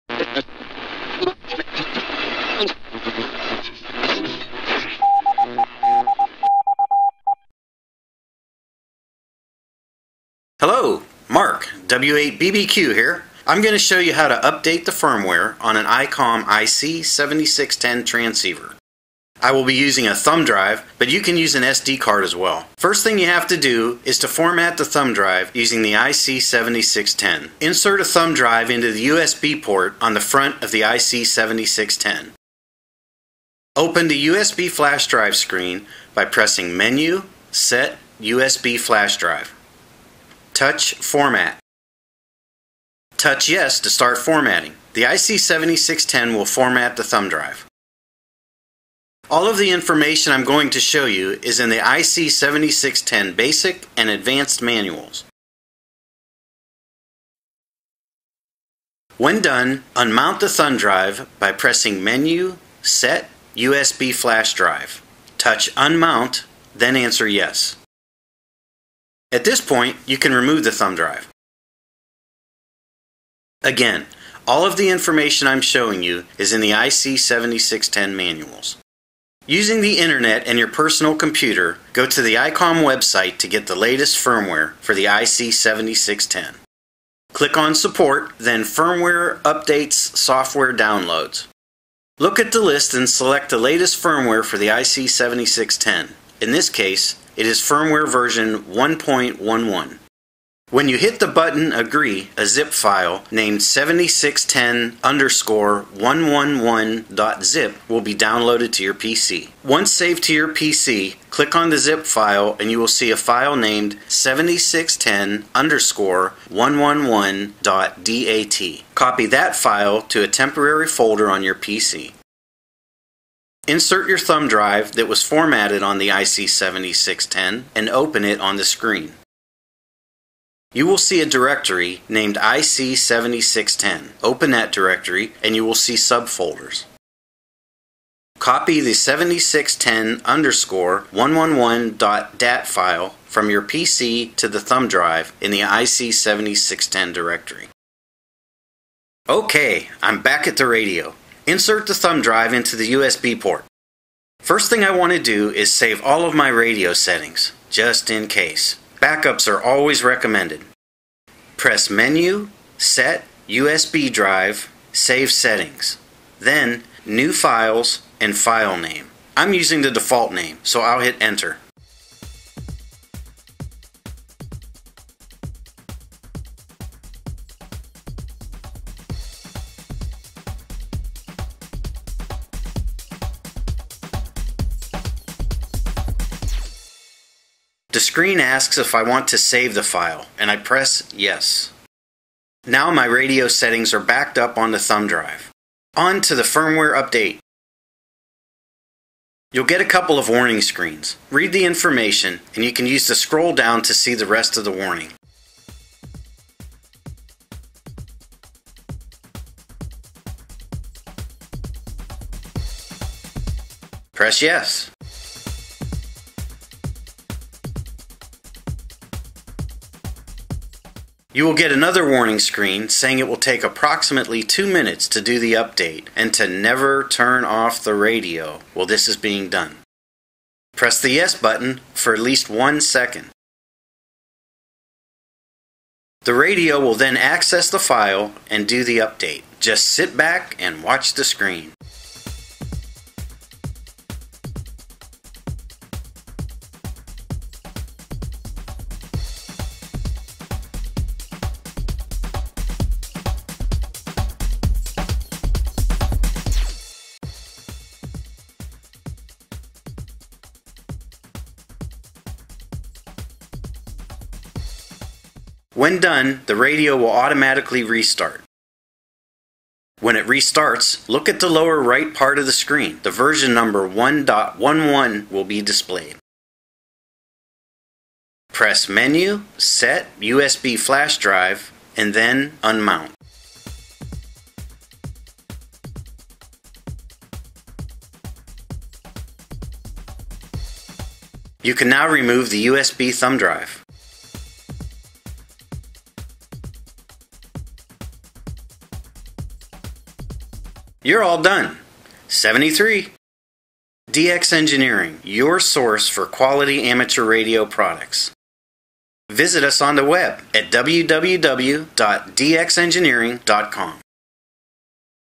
Updating the Firmware - Icom IC-7610
Updating the Firmware on an ICOM IC-7610 to version 1.11.

Step by step instructions showing how to update the firmware using a USB thumb drive.